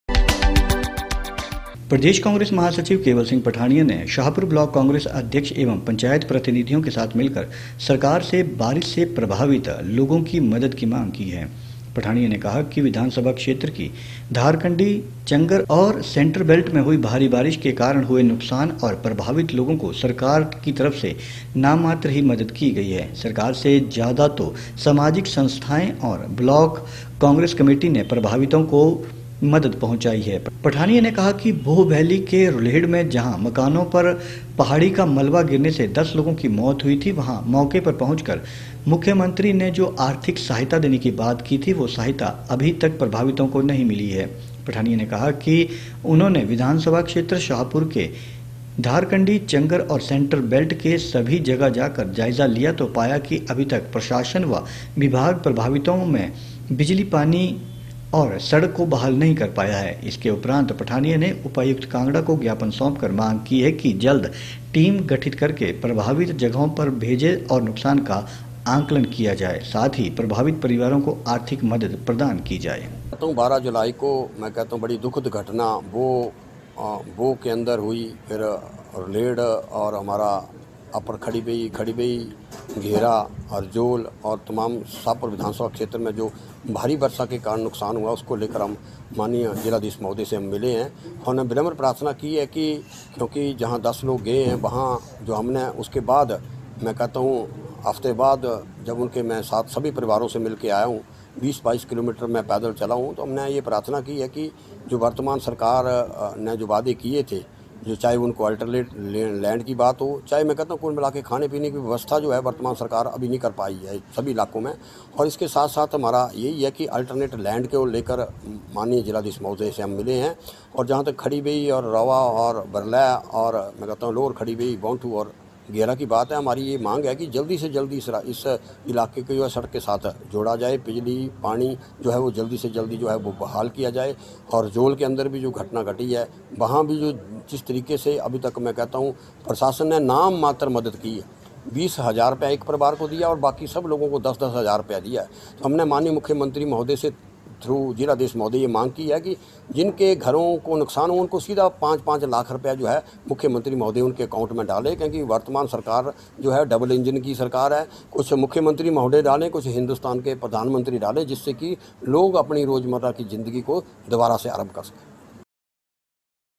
kangra कांग्रेस नेता केवल पठानिया ने उठाई मांगरुलेहड़ में प्रभावित परिवारों को नहीं मिली मदद
प्रदेश कांग्रेस महासचिव केवल सिंह पठानिया ने शाहपुर ब्लॉक कांग्रेस अध्यक्ष,
एवं पंचायत  प्रतिनिधियों के साथ मिलकर सरकार से बारिश से प्रभावित लोगों की मदद की मांग की है। पठानिया ने कहा कि विधानसभा क्षेत्र के धार कंडी,चंगर और सेंटर बैल्ट में हुई भारी बारिश के कारण हुए नुकसान और प्रभावित लोगों को सरकार की तरफ से नाममात्र मदद की है। सरकार से ज्यादा तो सामाजिक संस्थाओं और ब्लॉक् कांग्रेस कमेटी ने प्रभावितों की मदद की है। पठानिया ने कहा कि वोह वैली के रुलेहड़ में जहां मकानों पर पहाड़ी का मलबा गिरने से 10 लोगों की मौत हो गई थी, वहां मौका पर पहुंचे सीएम जयराम ठाकुर ने जो आर्थिक सहायता देने की बात कही थी, वो सहायता अभी तक प्रभावितों को नहीं मिल पाई है।